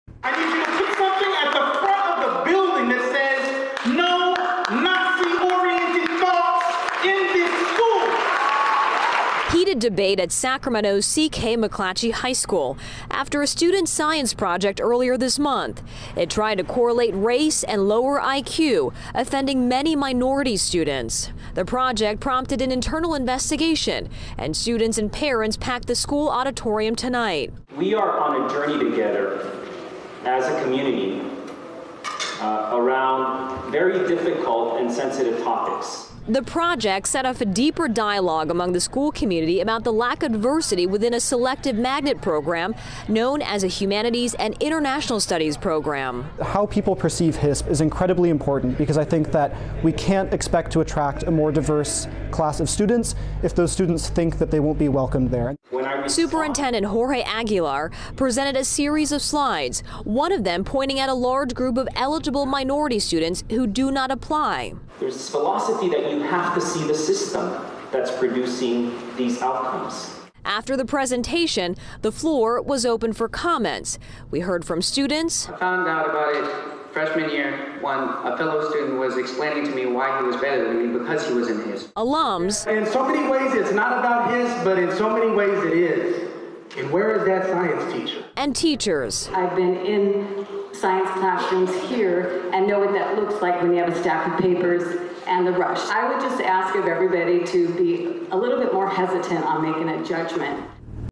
McClatchy HS discusses race after controversial science project
Hundreds packs a Sacramento school's auditorium Wednesday night to discuss race relations. Students, families and teachers were at McClatchy High School to talk about ways to prevent racism after a controversial science project targeted minority students. School leaders said they are looking at ways to change systems attached to their special programs.